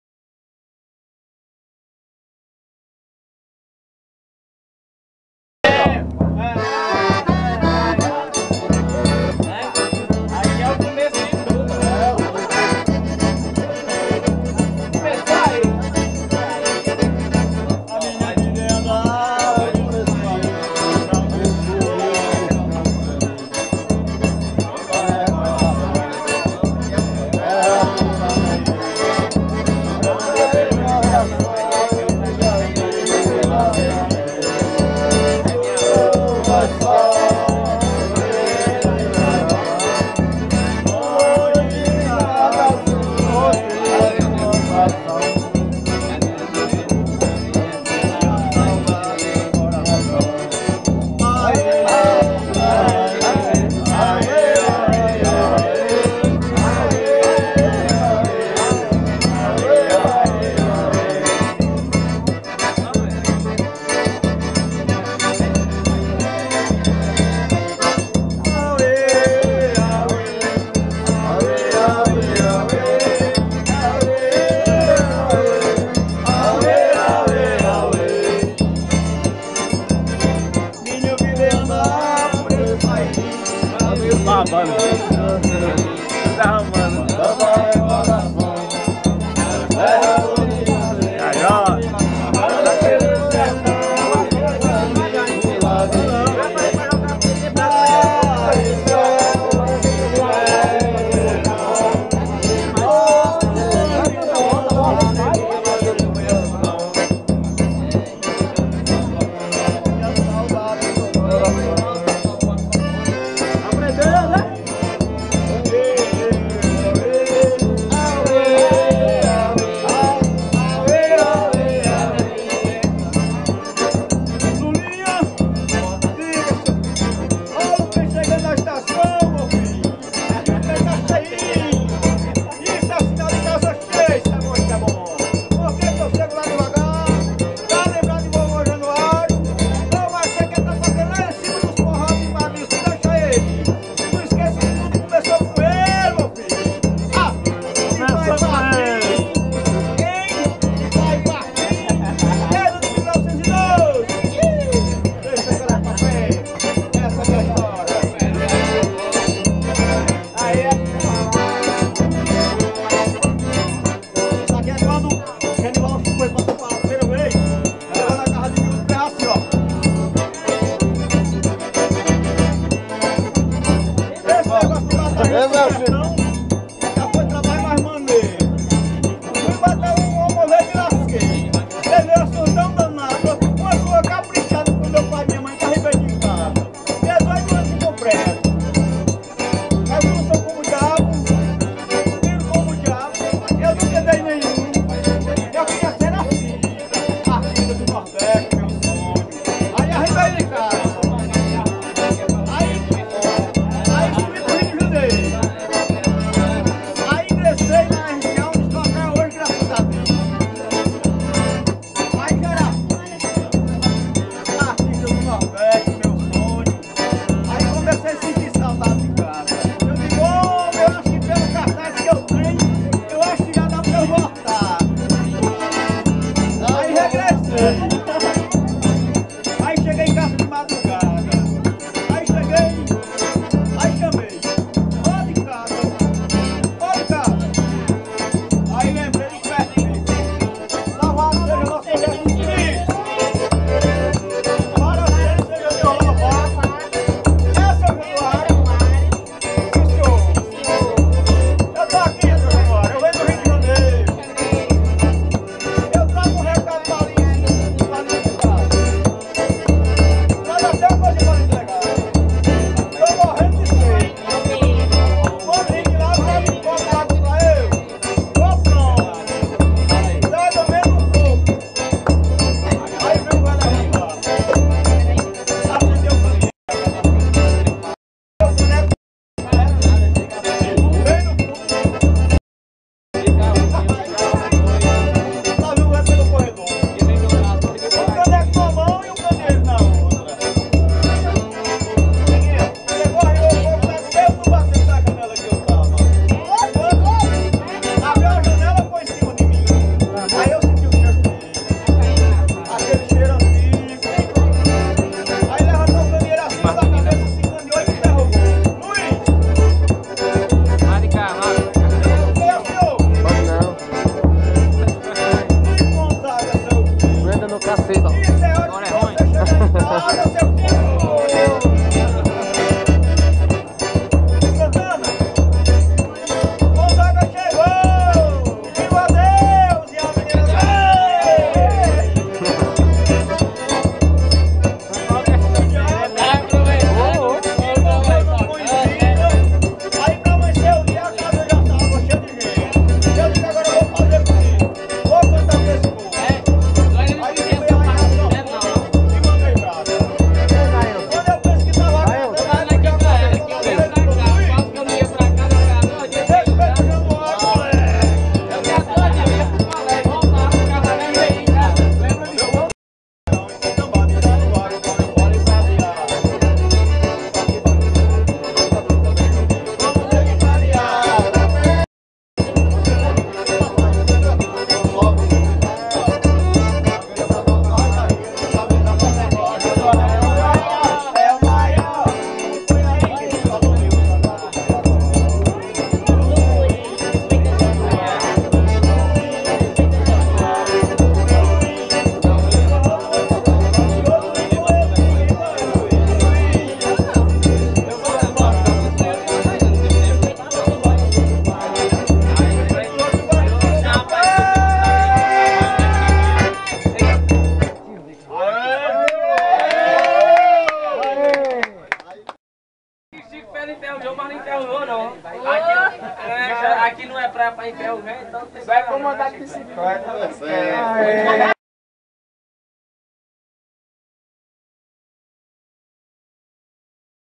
1° Part Pernambucanos Arretados Chico Felix, Paulo Felix, Nelson Felix,
Os Felix  De Bodocó-Pe. *CHICO FELIX, *PAULO FELIX, *NELSON FELIX,
festa na casa do tio
MUITO BOM MEUS TIOS PRIMOS TOCAM PRA CARANBA